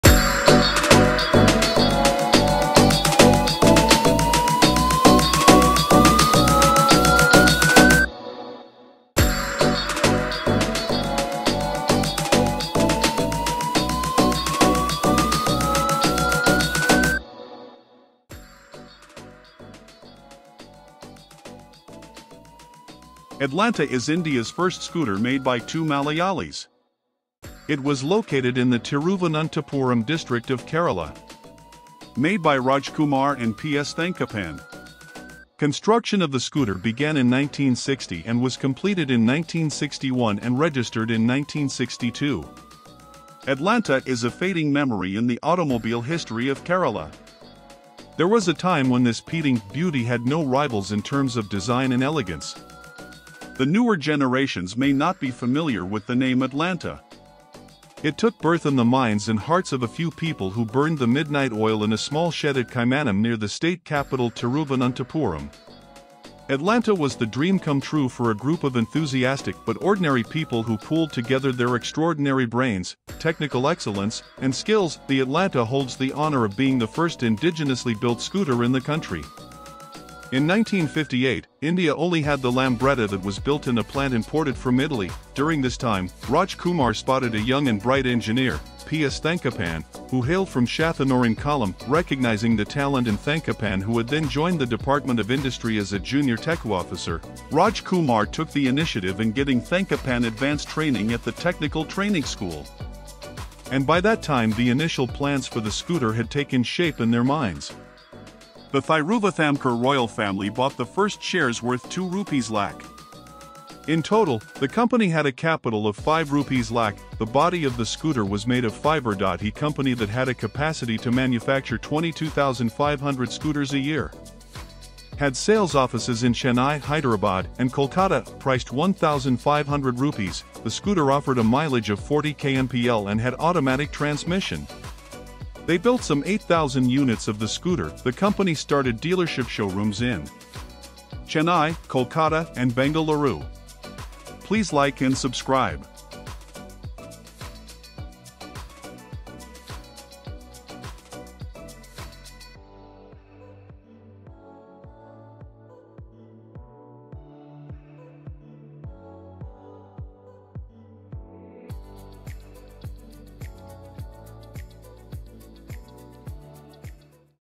The Legendary Automobile History of Kerala's Atalanta Scooter.
Welcome to  ZippyZapZoom  from Steam to Speed - Your Ultimate Destination for Everything Automotive ! The Legendary Automobile History of Kerala's  Atalanta Scooter. Atlanta is India's first scooter made by two Malayalees. It was located in the Thiruvananthapuram district of Kerala.
Made by rajkumar and PS thankappan. Construction of the scooter began in 1960 and was completed in 1961 and registered in 1962.

Atlanta is a fading memory in the automobile history of Kerala. There was a time when this "speeding..beauty" had no rivals in terms of design and elegance.

The newer generations may not be familiar with the name 'Atlanta'. It took birth in the minds and hearts of a few people who burned the midnight oil in a small shed at Kaimanam near the state capital Thiruvananthapuram.
Atlanta' was the dream come true for a group of enthusiastic but ordinary people who pooled together their extraordinary brains, technical excellence, and skills.The Atlanta holds the honour of being the first indigenously
built scooter in the country.